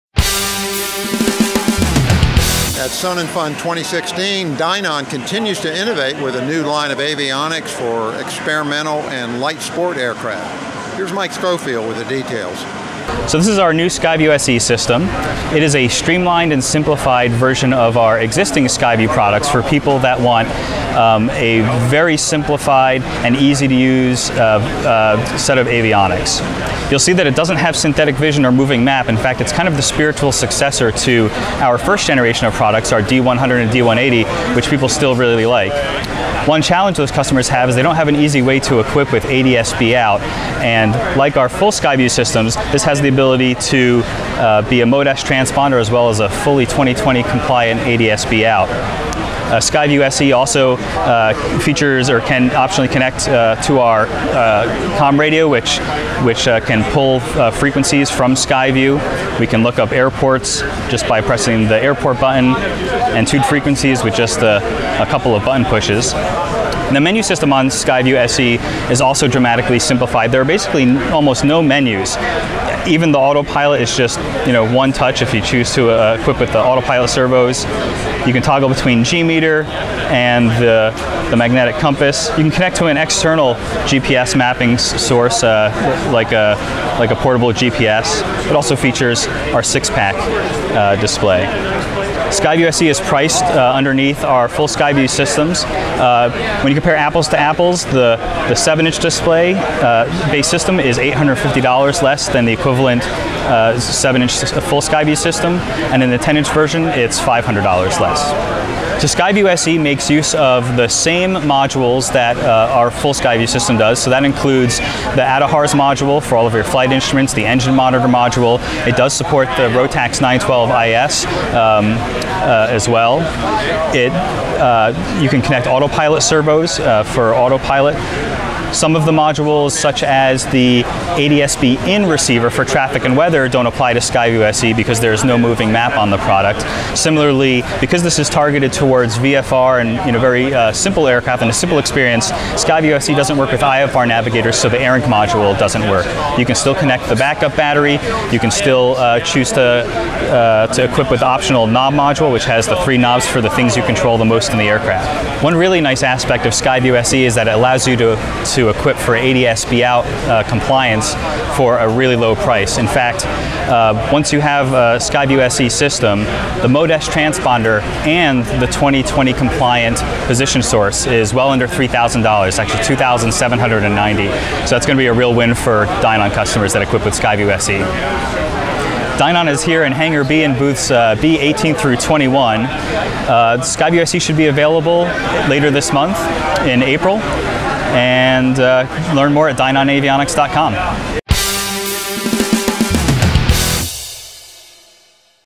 Dynon's New Skyview SE
At Sun 'n Fun 2016, Dynon continued to push into the world of non-certified avionics with its Skyview SE, a less expensive version of its popular Skyview EFIS system.  For "Kitplanes" magazine, Paul Bertorelli prepared this video report.